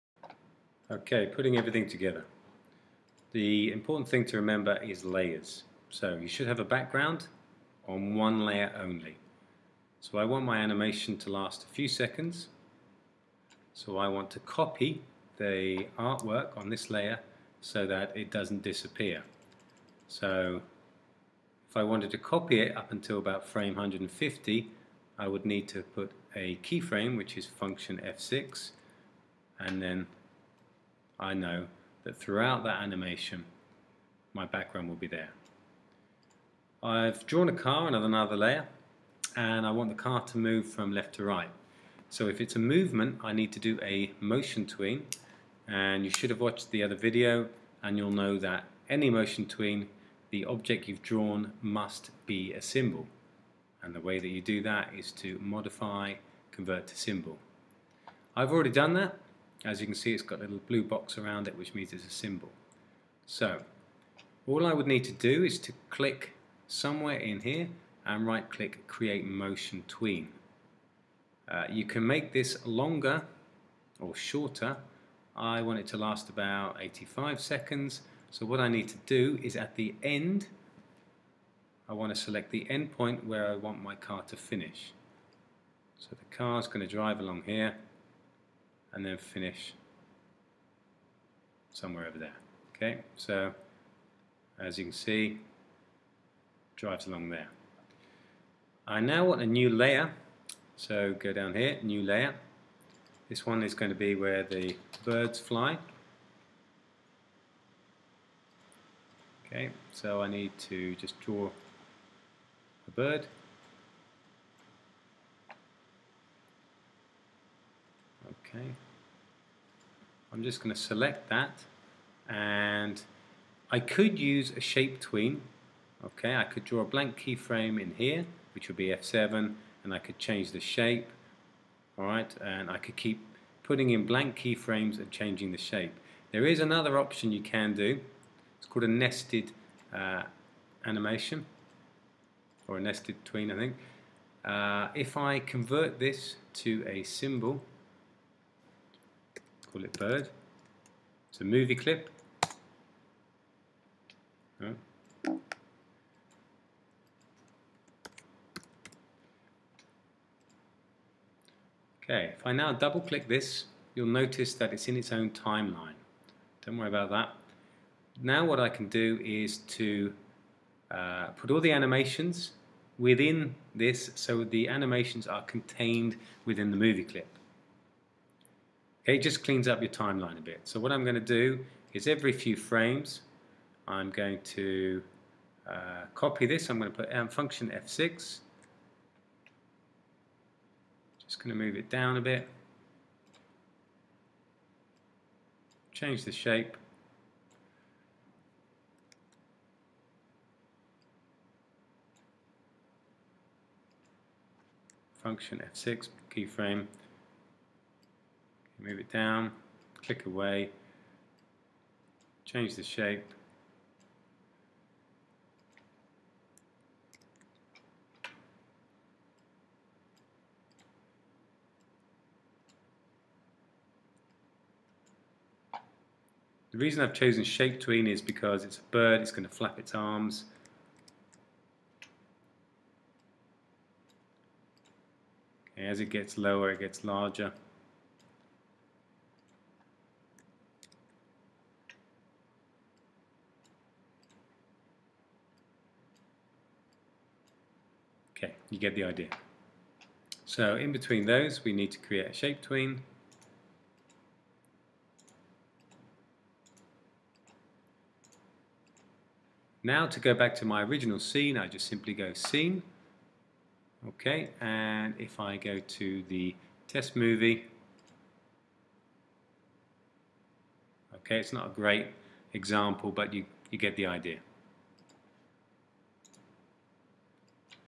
nested animation
Using layers and tweens to create an animation